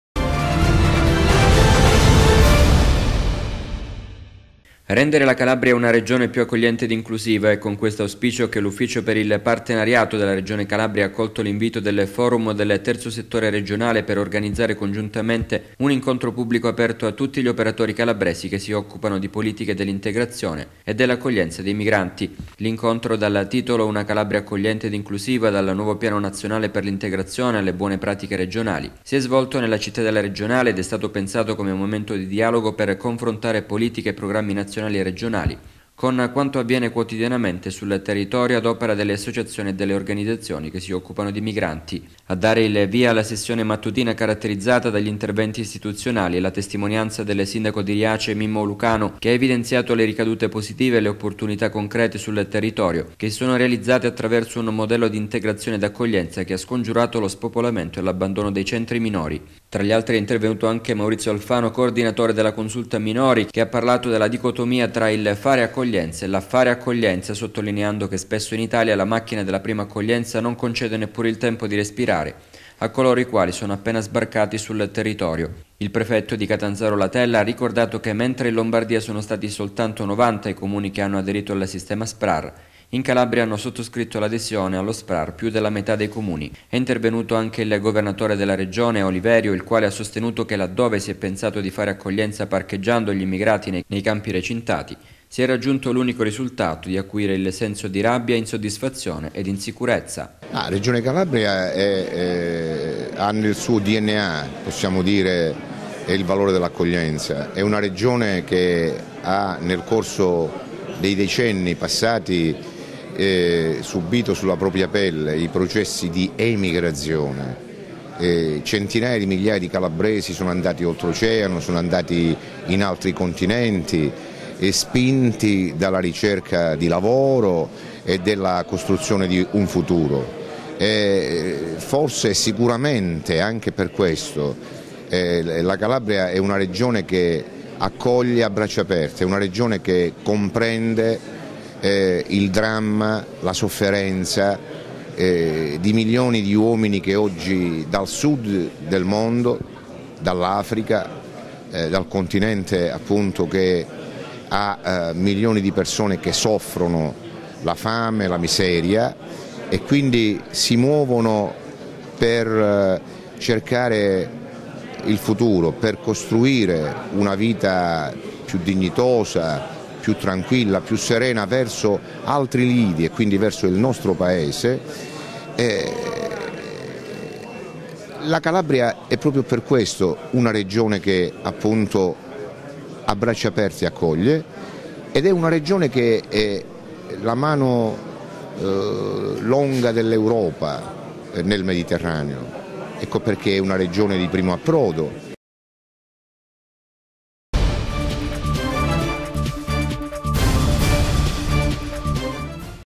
Immigrazione/ Confronto per una Calabria accogliente ed inclusiva RTC TELECALABRIA
Intervista a Mario Oliverio (Presidente Regione Calabria)